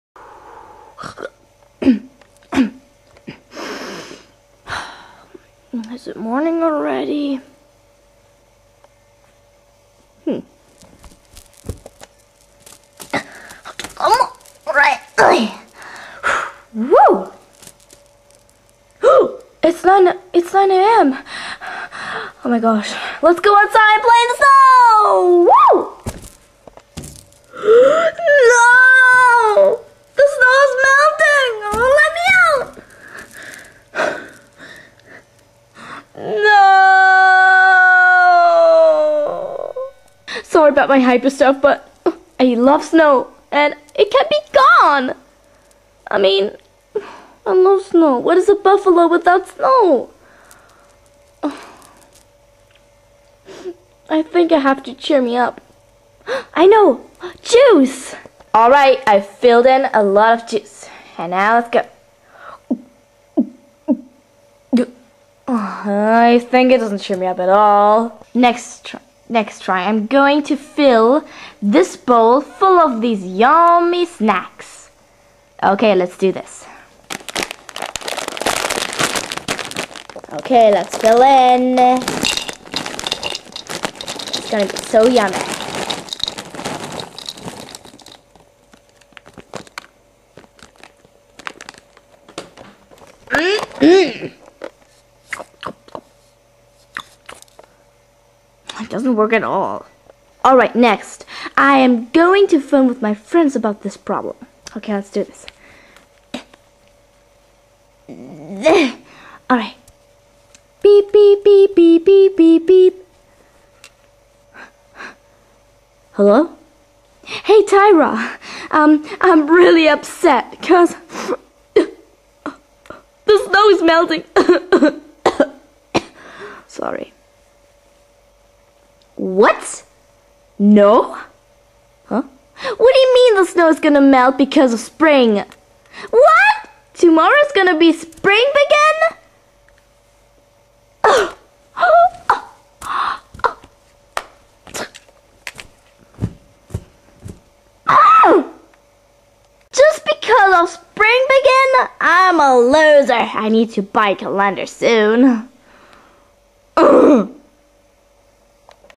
Webkinz Buffalo in Spring time
Rocky (my webkinz Buffalo)
finds out that snow is melting. He gets upset and wants to make him happy again!
Watch and learn .. or... not?